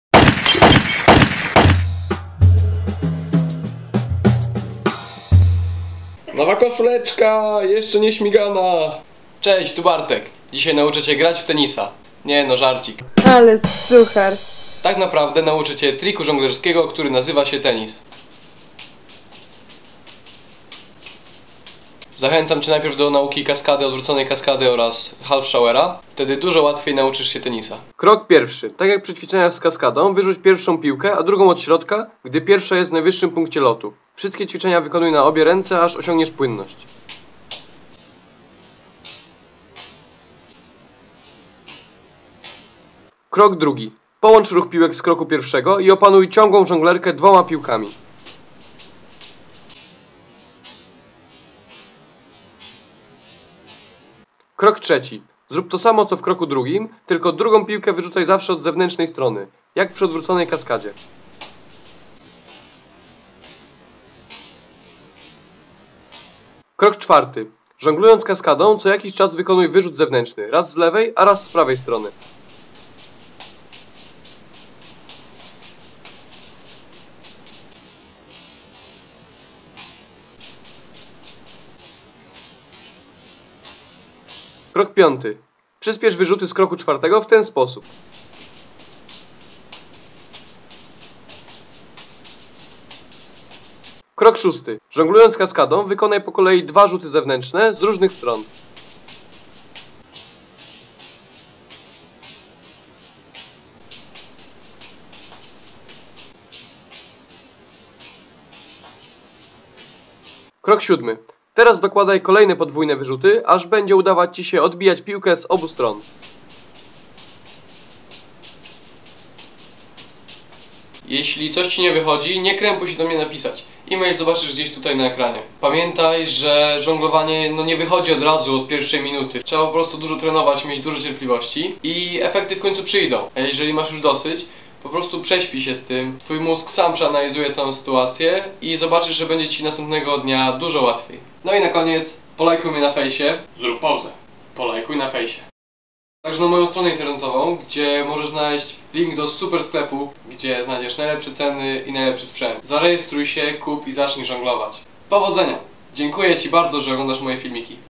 #5 Tenis - Tennis juggling tutorial in 7 steps - 7 kroków - Jak nauczyć się żonglować 3 piłkami
Powodzenia !

Copyright by Bartosz Bilejczyk, 2014.